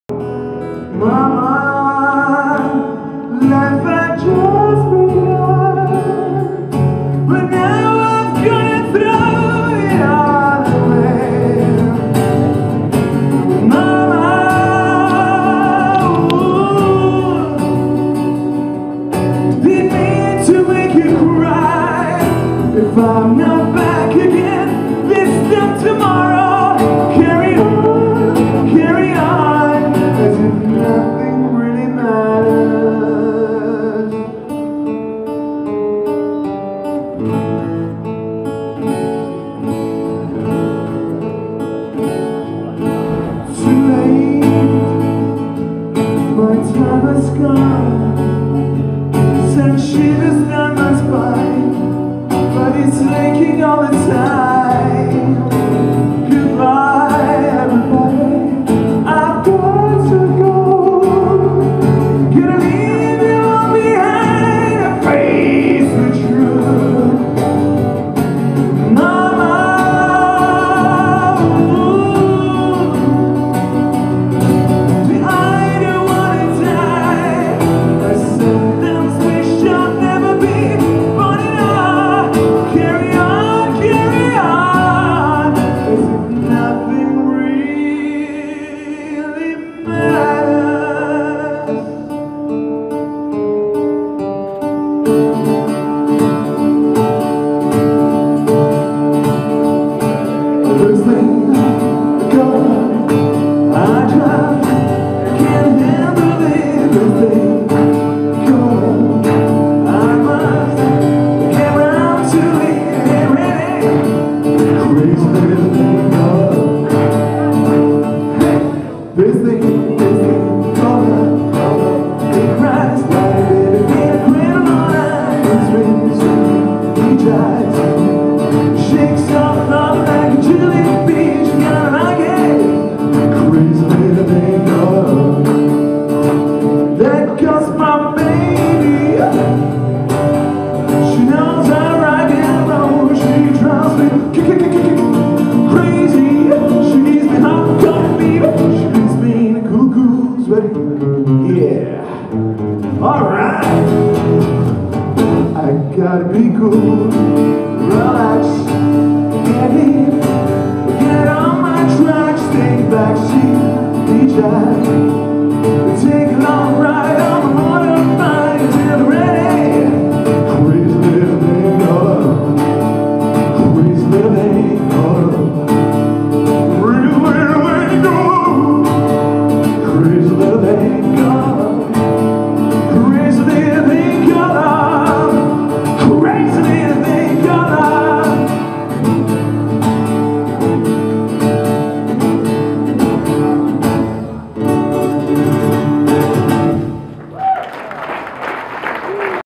luigi lusi apre il concerto di Stanley Jordan al Crossroads Live Club -15/2/2013
medley acustico dei Queen